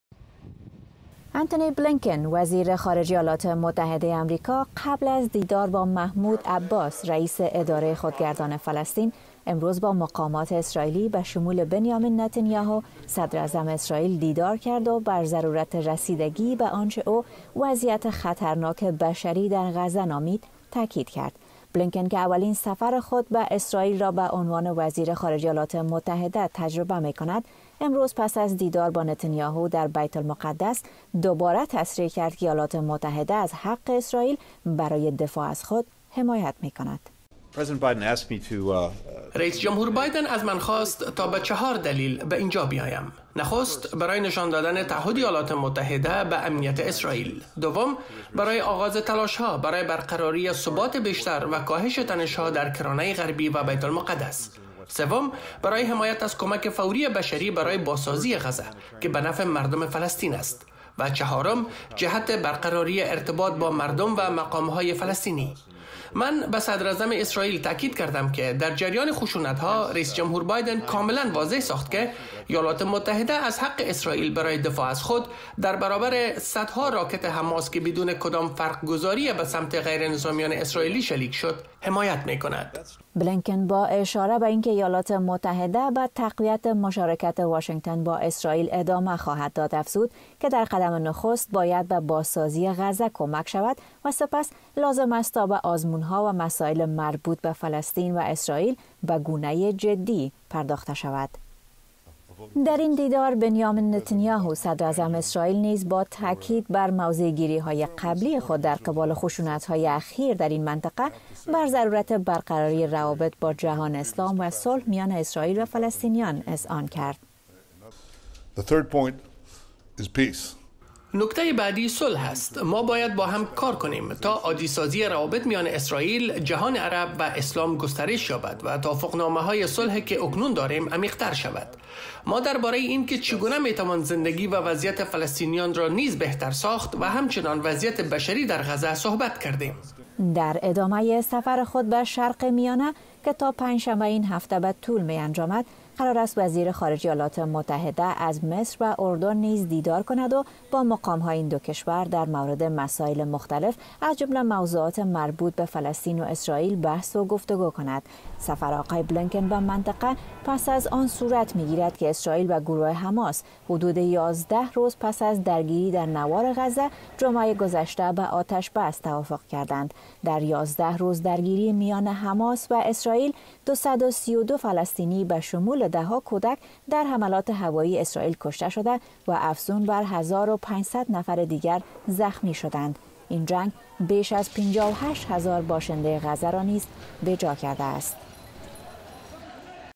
بلینکن بر ادامۀ آتش‌بس و کمک به غزه تاکید کرد
انتونی بلینکن، وزیر خارجه ایالات متحده امریکا، در سفرش به شرق میانه گفته است که واشنگتن در نظر دارد ۷۵ میلیون دالر را برای کمک های بشری به فلسطینیان اختصاص دهد. آقای بلینکن این سخنان را امروز در دیدار با محمود عباس، رییس اداره خودگردان فلسطین ابراز داشت.  قبل از دیدار با مقام های فلسطینی، آقای بلینکن با صدراعظم و شمار دیگری از مقام های اسراییل نیز ملاقات کرد.